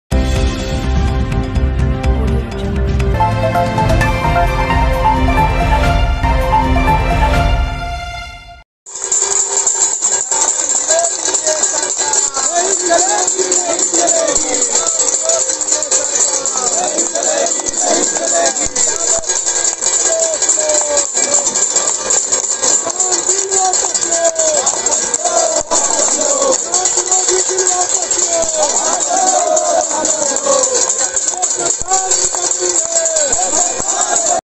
Tali-Thali Bajao Protest:किसान आंदोलन PM Modi के खिलाफ युवाओं ताली-थाली बजाकर किया प्रदर्शन
किसानों ने अब अंतरराष्ट्रीय स्तर पर सरकार की घेरेबंदी की तैयारी भी शुरू कर दी है
Oscar News Pvt. Ltd.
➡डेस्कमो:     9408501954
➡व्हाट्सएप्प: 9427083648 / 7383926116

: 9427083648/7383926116